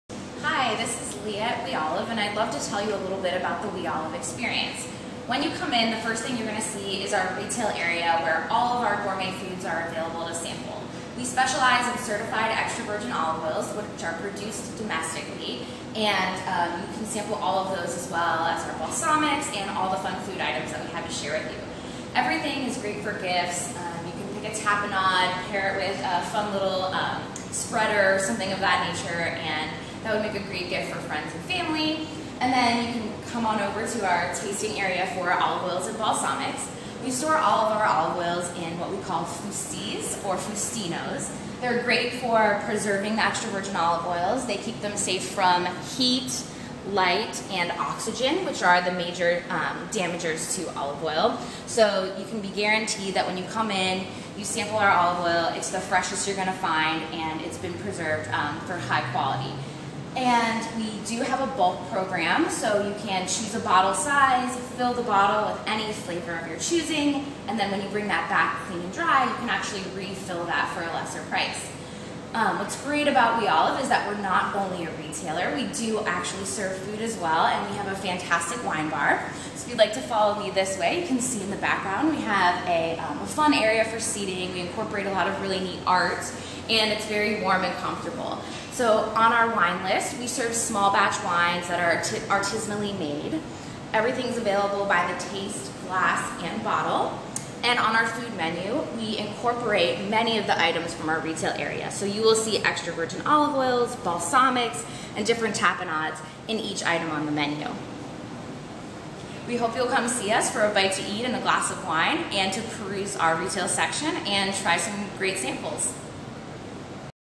We Olive & Wine Bar Cincinnati Tour
We Olive & Wine Bar is all about tastings so Leah Jones is giving you a little taste of the We Olive Experience.